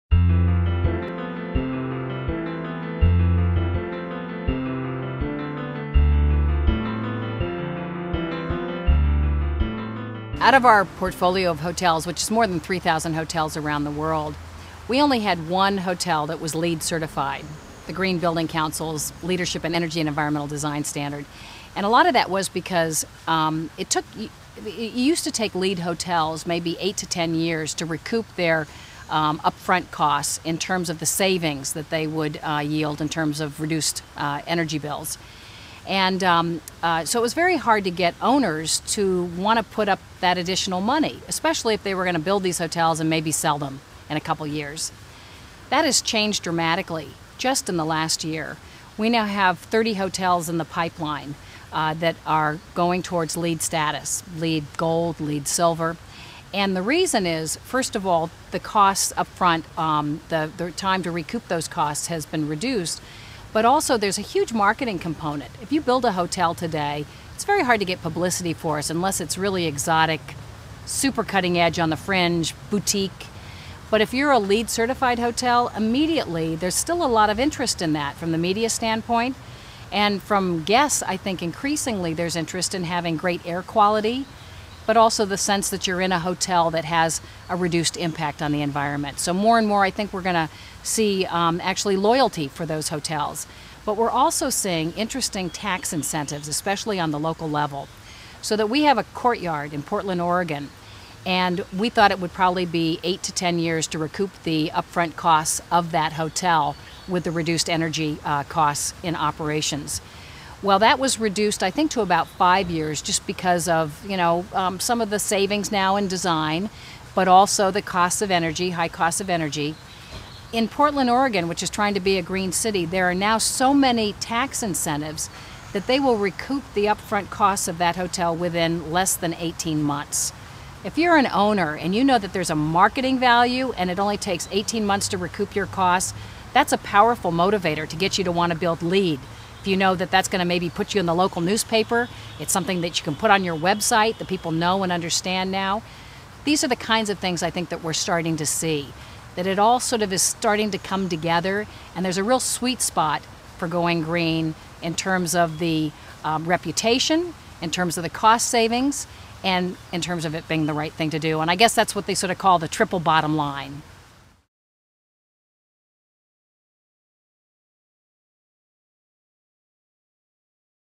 Kathleen Matthews for Marriot Internationals - Advantages of building according to LEED
Interview with Kathleen Matthews, Marriott International's Executive Vice President Global Communications and Public Affairs.

Building according to LEED standards doesn't have to be more expensive!

the video site for sustainable business intelligence.